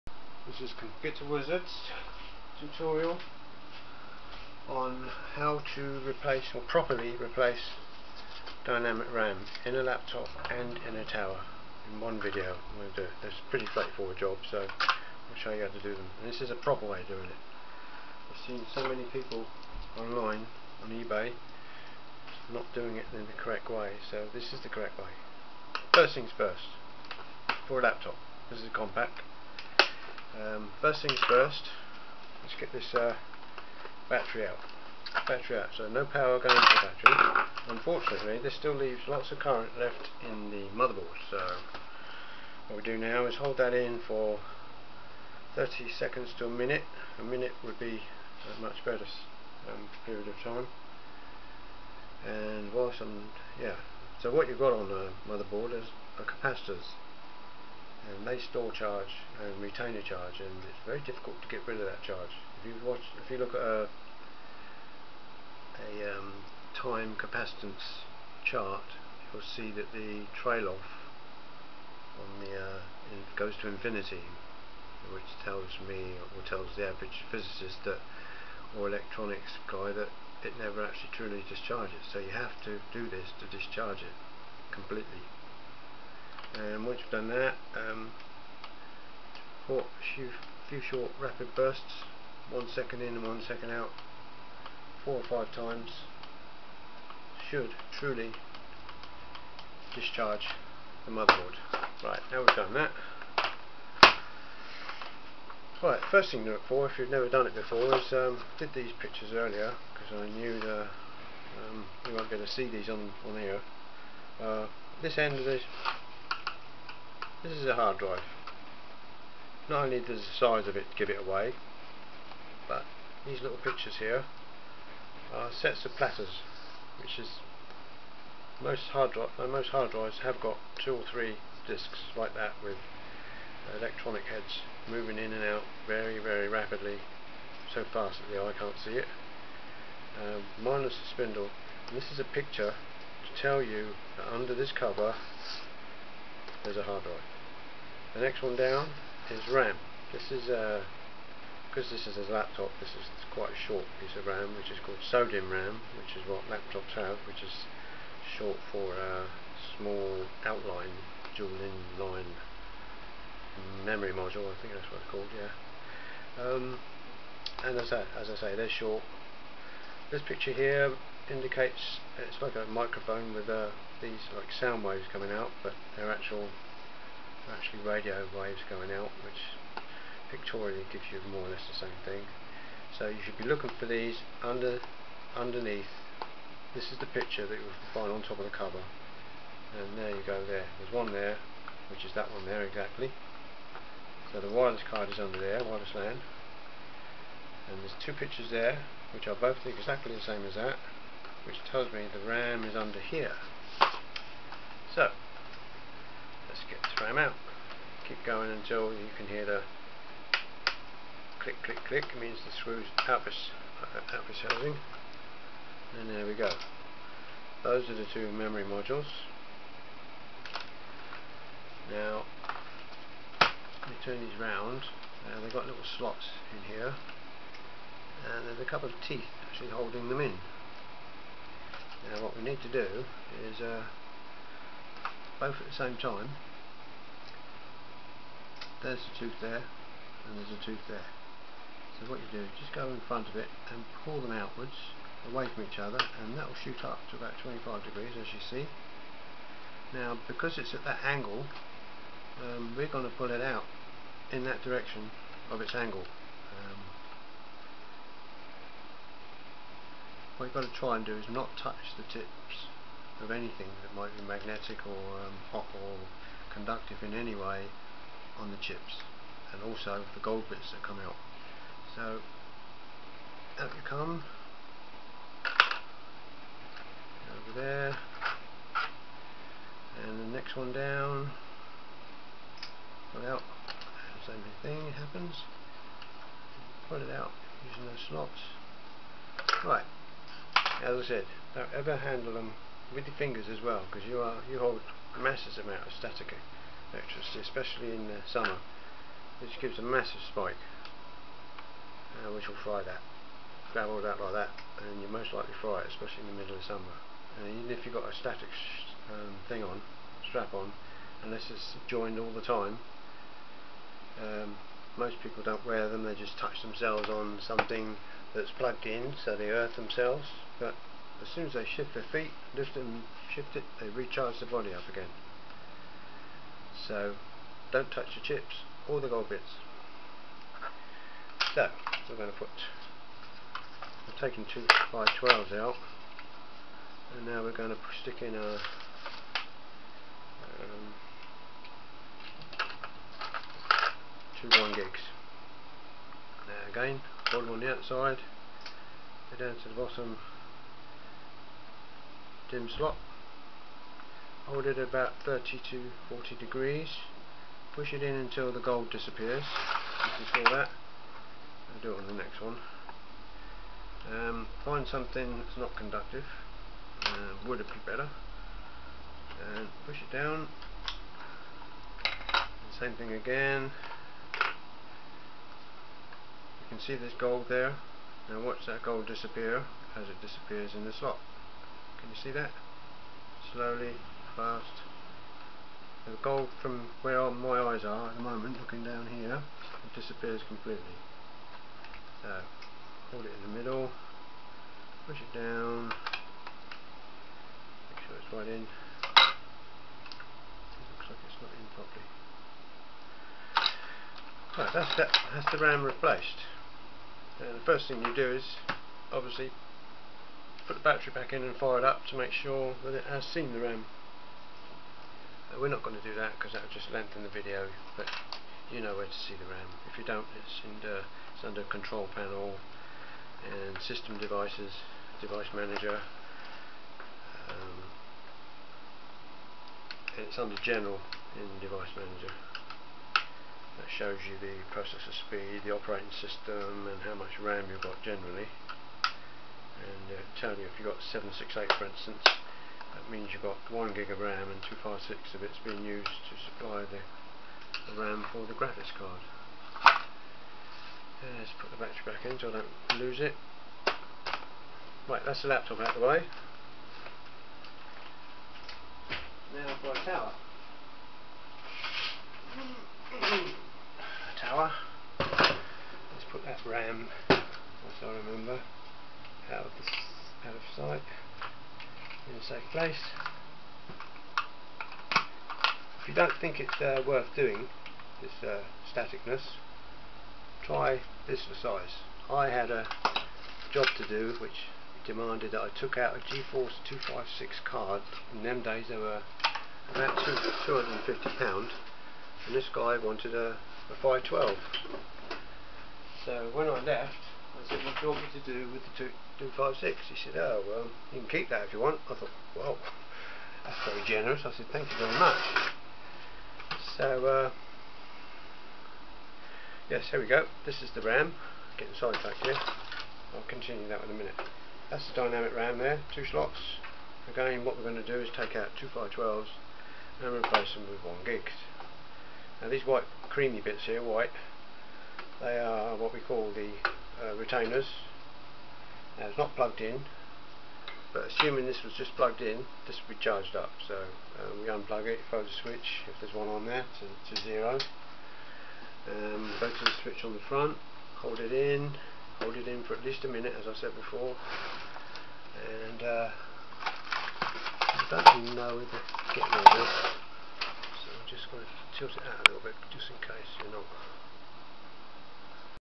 how2 'properly' replace or upgrade dynamic ram in a laptop
hey girl'z n' boy's, jakkadan here again, i would like to show you how2, 'properly', change or increase the dynamic ram, (ram is short for 'random access memory) in a Laptop and a PC tower in this, 'how2 fix laptop's n' pc's 4 dimwits', computer repair video tutorial.
However, this video ends before the changing of the ram module in the PC, so i have since uploaded the full, Laptop 'and' PC, (16 minute) version that it was supposed to be, That's what happens when you don't check ur video out before you put out, eh??.
Your 'dynamic' ram is a memory module in your computer where data is temporarily stored, while it is being manipulated by or 'gets' accessed by the (cpu), the central processor unit and then once what ever task was been completed, it is then passed to the hard drive for permanent storage.
When you shut down your computer, the dynamic gets 'flushed', emptied, so all data still resident is lost.
A programming language organises all these transactions which is the software called the operating system.
This set of notes and instructions can be used alone but for an increased understanding it would better used in conjunction with this associated computer repair video tutorial, 'how2 'properly' replace or upgrade dynamic ram in a laptop'.
I have watched so many 'help' videos by people who purport to have the knowledge and expertise to give instruction on how2 change potentially delicate components, such as; ram, video cards, screens, etc.
Yet almost everyone of them miss out these next set of crucial steps to avoid 'frying' or shorting out their components, either by inherent left over current in the motherboard or by static discharge, , hence the reason for this video.
With this in mind, before we do anything it is extremely important to remove every, amp. and micro-ampere of current; so first for laptops, netbooks.etc, the battery must be removed and then make sure you remove all leads plugged into the mains, or any other cables plugged into it
Only now are we ready 2 remove the ram. The ram will be more than likely to be underneath the laptop under a cover, nearly every newer type laptop has one of these underneath, it will be about 50mm x 90mm or there will be a very large cover which covers the ram, cpu and maybe even the hard-drive too. they will be marked with a little pictures of a memory module, a series of disks with the spindle removed or wlan 'aerial' symbol, there will also be a small fan aperture, see video.
Unscrew the memory module cover retaining screws, once u have located the ram make a note of the module's retaining 'lug's, (they are at each end of each module), and find a couple of long thin rods something like two short knitting needles, or chop sticks would be ideal, as these are wood or similar non conducting material.

Locate the retaining 'lug's at each end of the memory modules, see video and by using the tips of the 'rod'z, slide them away from each other and the ram will move away from the motherboard to a 25-30 degree angle.
Now just before we move on be very careful not to touch any of the memory module chips or the gold connector pins when handling, the old ram and especially the new ram and that definitely means without ur bare fingers, u carry a huge 'static' charge around with u in ur body .
Using the rod'z  u can also easily push/pull out the ram module/s out, in the direction in which it they are pointing ie: perpendicularly to that angle, again see the video for more clarity. there will be two slots, staggered in their location.
You will have already worked out what ram stick/s u require to have to put bcak in, laptop ram is called SO-Dimm, which is short for 'small-outline dual in memory module'. try to have the new ram close by ready to install and when replacing the new so-dimm module hold it at about 25-30 degrees to the work bench, and push it into the module's slot with a little force in-line with it's spine through to it's gold pins, with the row of gold pins almost disappearing into it's slots, then with something non conductive and non-metallic, (a pencil with a rubber on the end would be perfect), flatten the ram from30 degrees through to zero degrees, parallel to the bench top, again see the accompanying video.
Put battery back in or the charger lead and fire it up if you get a single short beep, then the ''power on self test'' was successful and new ram has been detected, hold in the button and shut it down again and replace covers. congratulations you have now upgraded ur ram

voila!!! ur done, qed!!

Author Peter Edyvane
For How2 fix laptop's n' pc's 4 dimwits